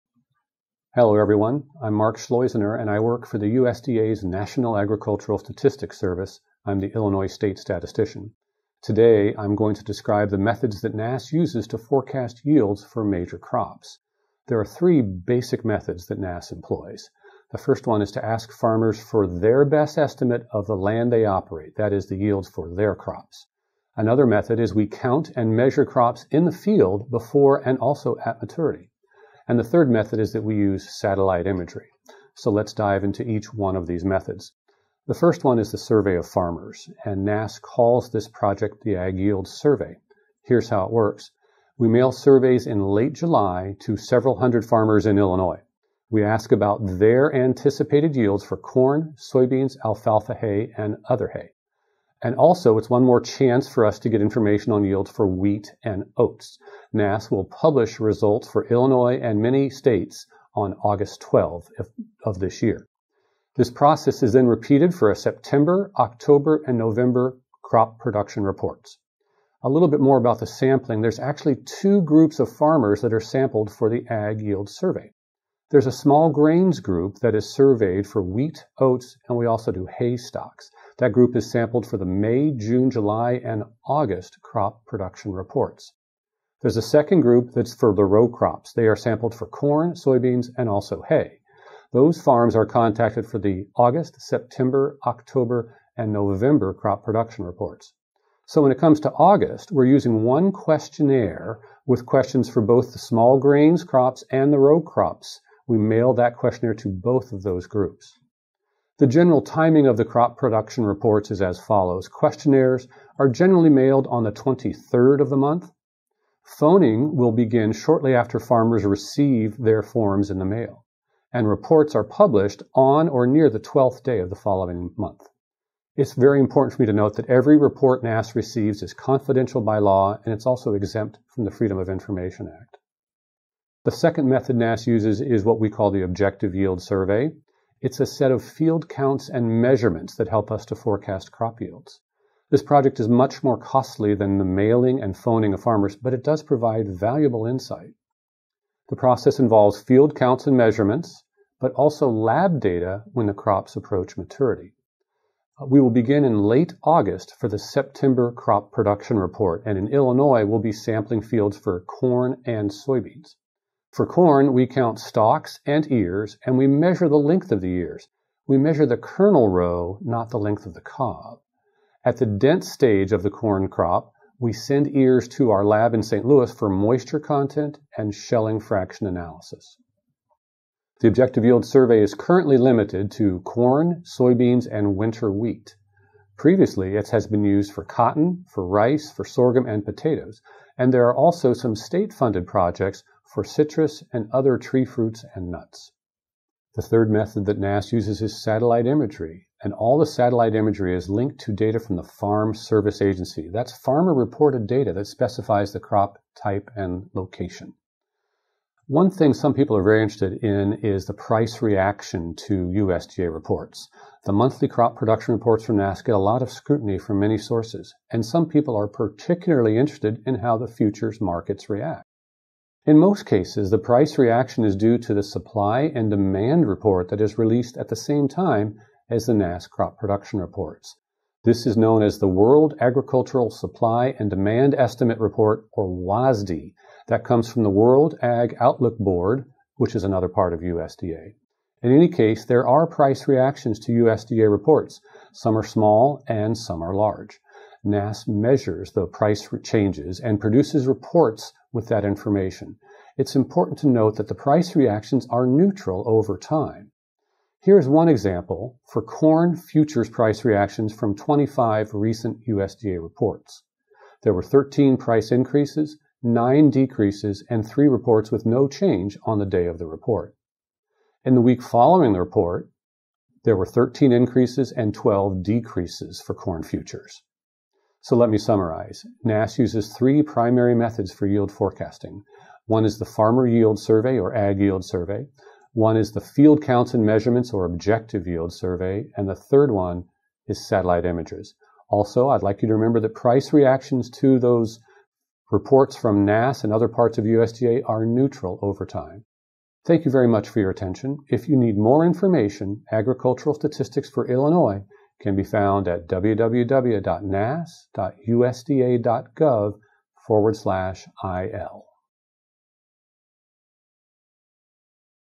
NASS Yield Forecasting Methods
Dive into the world of agricultural forecasting with Mark Schleusener, Illinois State Statistician for USDA's National Agricultural Statistics Service (NASS). This video unveils the three primary methods NASS uses to predict crop yields: farmer surveys, field measurements, and satellite imagery. Learn about the AG Yield Survey, Objective Yield survey, and how NASS utilizes cutting-edge technology to provide accurate crop production reports. Discover the impact of these reports on futures markets and gain valuable insights into the complex world of agricultural statistics. Whether you're a farmer, trader, or ag enthusiast, this video offers a comprehensive look at how USDA forecasts shape the agricultural landscape.

0:00 - Introduction
0:14 - Three Methods of Yield Forecasting
0:34 - Survey of farmers
2:12 - Objective Yield Survey Process
3:16 - Satellite Imagery in Forecasting
3:28 - Price Reactions to USDA Reports
4:37 - Conclusion and Resources